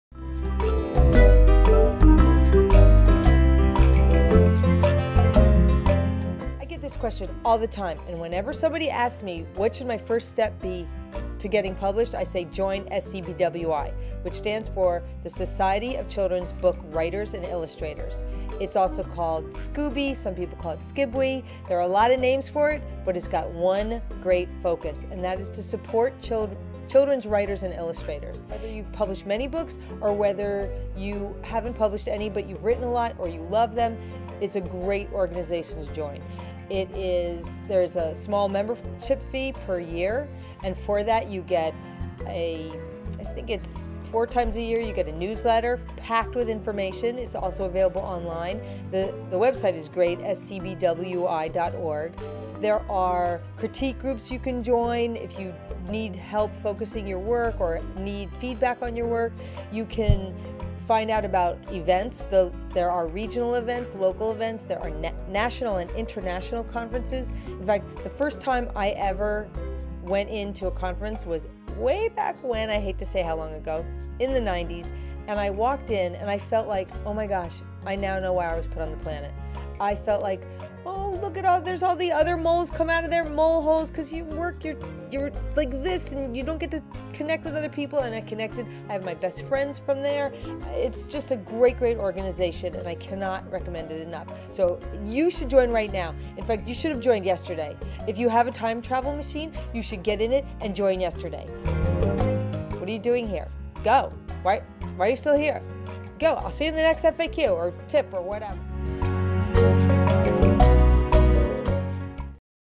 First Step to Getting Published
Ever wondered if you could publish a children's book? Author/illustrator Katie Davis joined SCBWI, the Society of Children's Book Writers and Illustrators when she was starting out and it was her first step to getting published by major publishers.